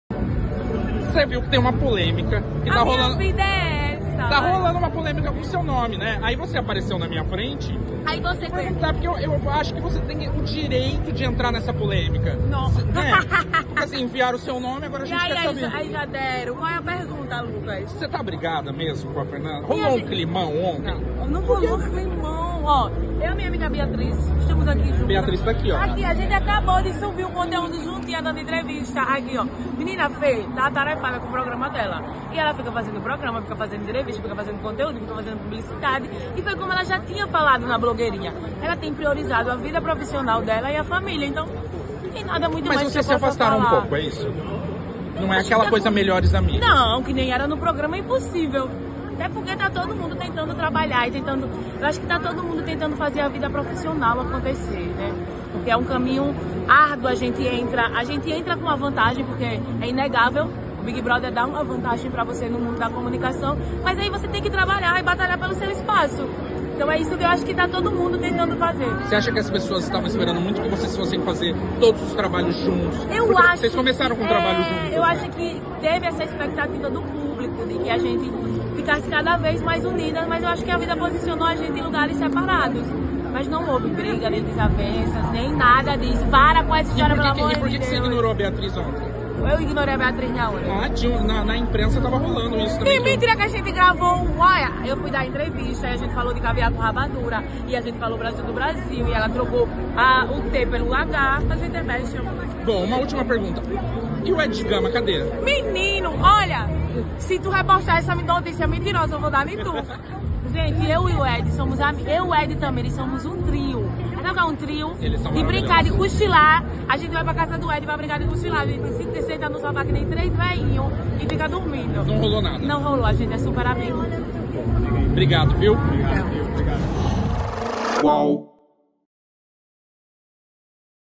Climão? Pitel reage sobre Fernanda após rumores de afastamento no Rock in Rio
A ex-BBB explicou que Fernanda Bande tem priorizado o trabalho e que de fato houve um afastamento entre elas.